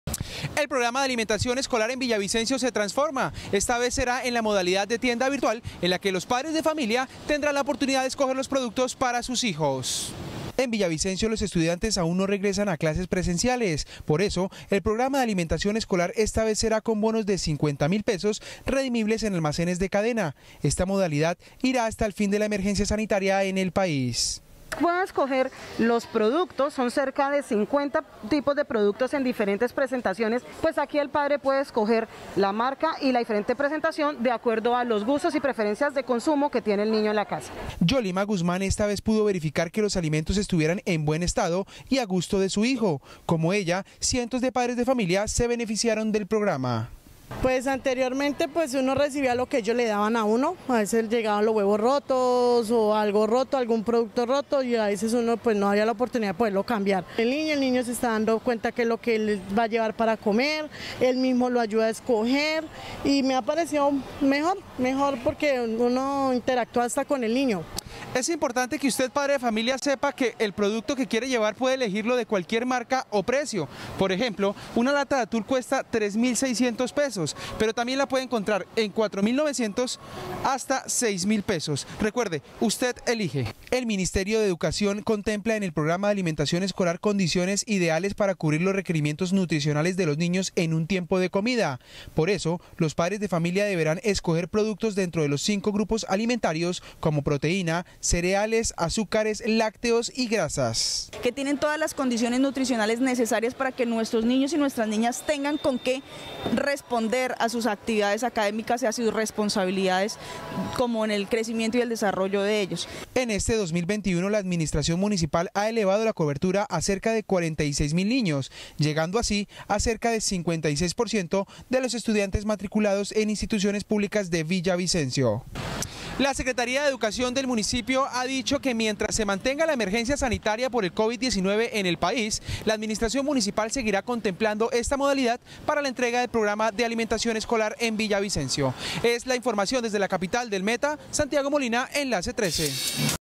Alimentación escolar en Villavicencio será a través de bonos redimibles | Noticias Enlace Trece
Alimentación para todos. En el departamento del Meta, el plan de alimentación escolar 🧃 se puede reclamar en bonos redimibles en almacenes de cadena con el fin de llegar a todos los estudiantes.